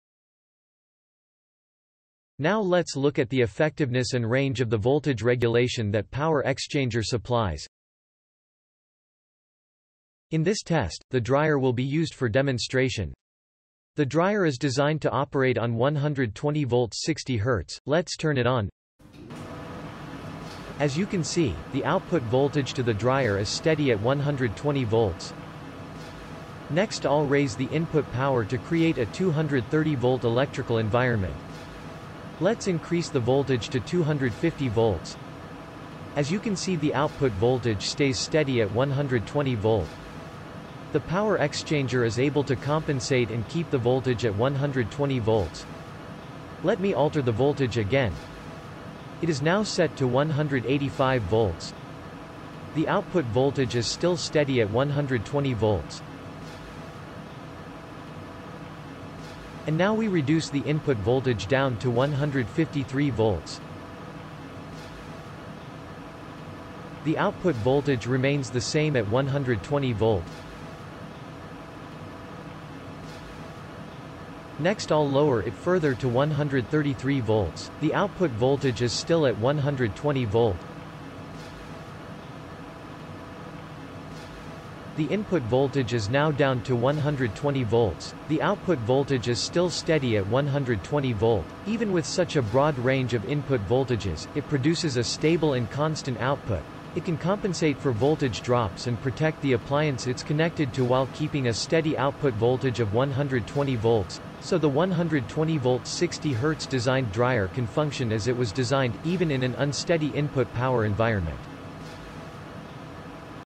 Demonstrating 220V / 50Hz to 120V / 60Hz, using a voltage and frequency converter
A PowerXchanger X-10, 10 amp unit is being used to run a 120 Volt 60Hz clothes dryer. Our chief engineer demonstrates how PowerXchanger technology can regulate a constant 120 volts 60 Hz output while input voltage fluctuates widely as high as 255 Volts and as low as 119 Volts

To Purchase: